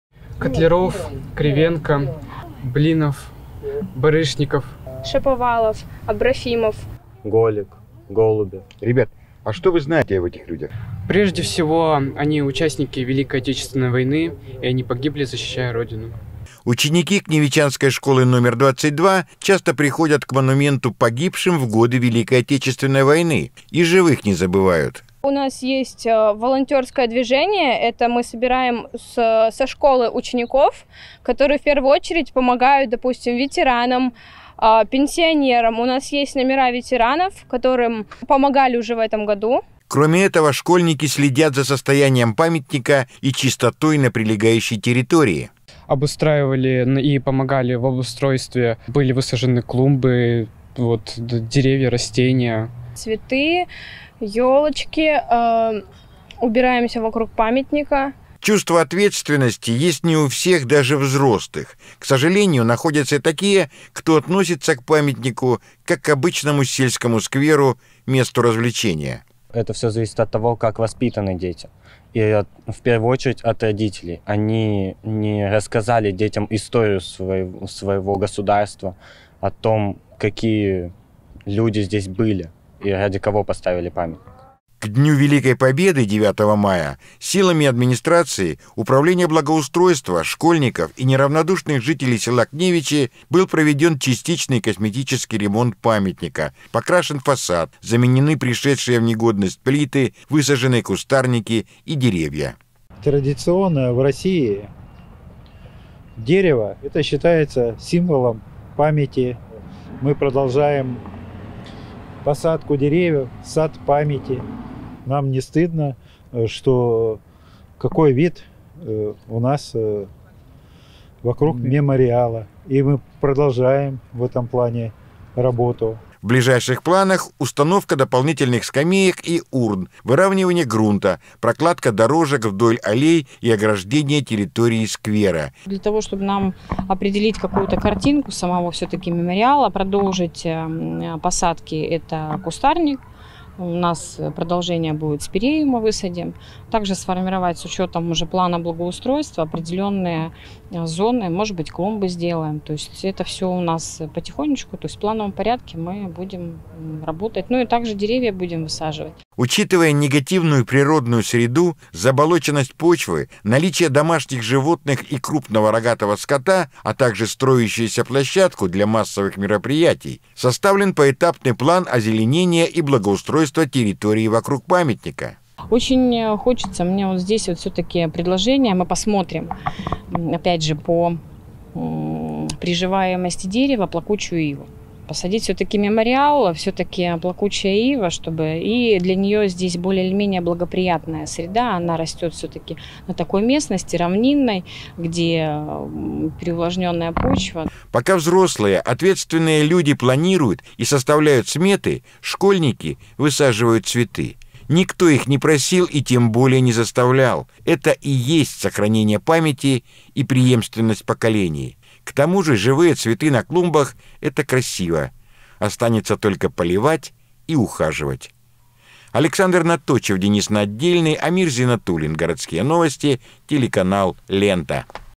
Пока мы помним
Ученики Кневичанской школы 22 часто приходят к монументу,  погибшим в годы Великой Отечественной войны.  Они понимают  ответственность перед старшим поколением и всеми силами стараются сохранить память о земляках, погибших на войне. И живых не забывают. Школьники  следят за  состоянием монумента и чистотой на прилегающей территории. Силами администрации, Управления благоустройства,  школьников и неравнодушных жителей села Кневичи, к 9 мая, был проведён частичный косметический ремонт памятника, покрашен фасад, заменены пришедшие в негодность плиты, высажены кустарники и деревья.